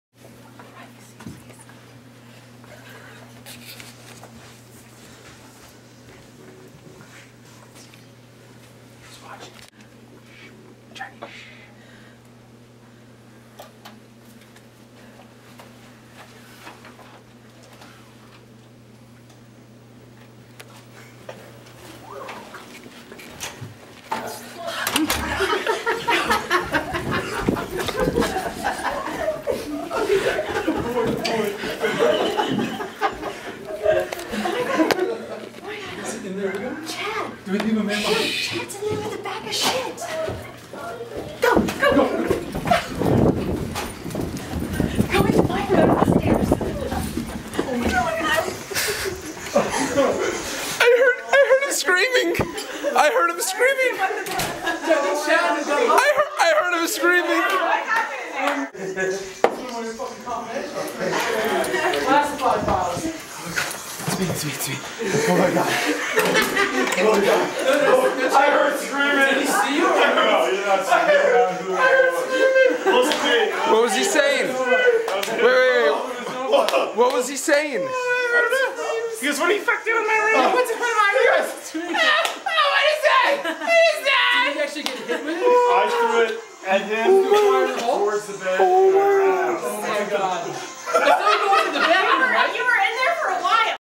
mr. shit pockets gets attacked by a bag of shit
mr. shit pockets sleeps.  mr. shit pockets gets attacked.  woe is all.